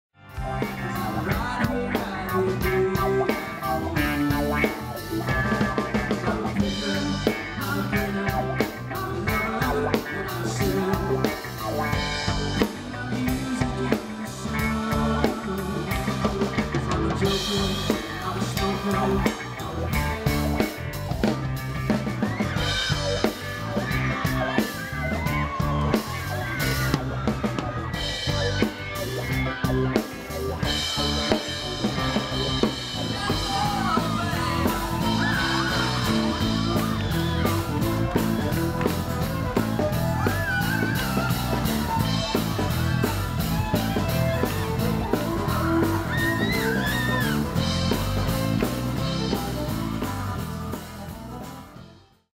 Carus & the True Believers on the 2008 Great Vic!
Carus and the True Believers rocked Halls Gap on the 2008 Great Victorian Bike Ride, people of all ages danced the night away to the tunes from this top Australian artist.